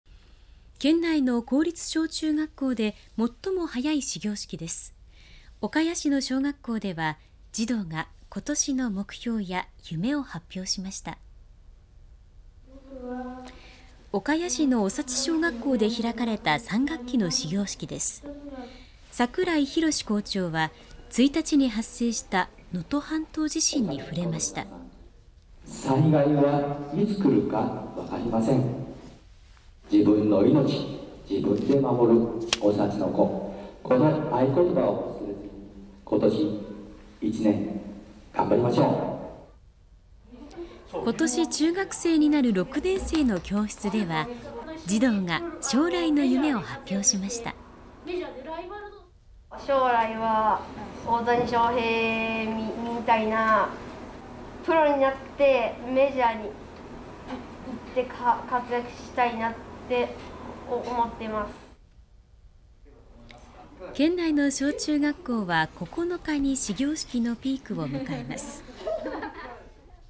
「災害はいつ起きるか分からない」 3学期スタートで児童に呼び掛け（abnニュース 2024.01.05）
県内の公立小・中学校で最も早い始業式です。岡谷市の小学校では児童が今年の目標や夢を発表しました。

岡谷市の長地小学校で開かれた3学期の始業式です。桜井洋校長は1日に発生した能登半島地震に触れました。

■長地小学校・桜井洋 校長
「災害はいつ起きるか分かりません『自分の命、自分で守る長地の子』この合言葉を忘れずに今年1年頑張りましょう」

今年中学生になる6年生の教室では、児童が将来の夢を発表しました。

■6年生
「将来は大谷翔平みたいにプロになってメジャーに行って活躍したい」

県内の小中学校は9日に始業式のピークを迎えます。